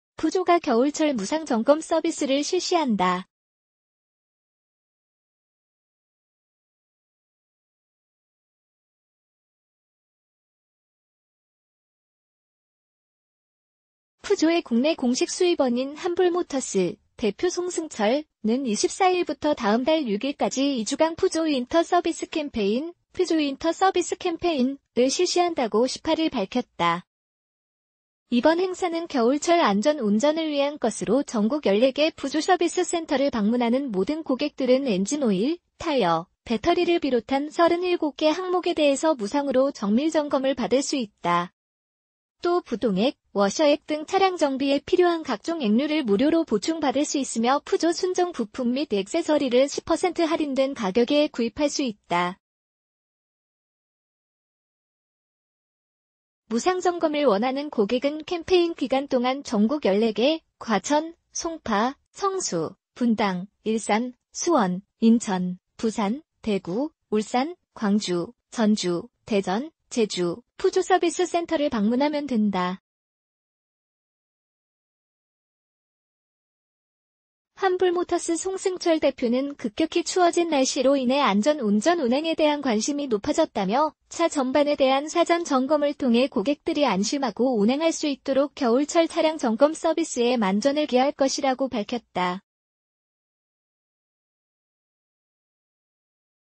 푸조, 겨울철 무상 점검 서비스 실시
푸조가 겨울철 무상 점검 서비스를 실시한다.

푸조의 국내 공식 수입원인 한불모터스(대표 송승철)는 24일부터 다음 달 6일까지 2주간 ‘푸조 윈터 서비스 캠페인(Peugeot Winter Service Campaign)’을 실시한다고 18일 밝혔다.

이번 행사는 겨울철 안전 운전을 위한 것으로 전국 14개 푸조 서비스 센터를 방문하는 모든 고객들은 엔진오일, 타이어, 배터리를 비롯한 37개 항목에 대해서 무상으로 정밀 점검을 받을 수 있다.

또 부동액, 워셔액 등 차량 정비에 필요한 각종 액류를 무료로 보충 받을 수 있으며 푸조 순정 부품 및 액세서리를 10% 할인된 가격에 구입할 수 있다.

무상점검을 원하는 고객은 캠페인 기간 동안 전국 14개(과천, 송파, 성수, 분당, 일산, 수원, 인천, 부산, 대구, 울산, 광주, 전주, 대전, 제주) 푸조 서비스 센터를 방문하면 된다.

한불모터스 송승철 대표는 “급격히 추워진 날씨로 인해 안전 운전 운행에 대한 관심이 높아졌다”며, “차 전반에 대한 사전 점검을 통해 고객들이 안심하고 운행할 수 있도록 겨울철 차량 점검 서비스에 만전을 기할 것”이라고 밝혔다.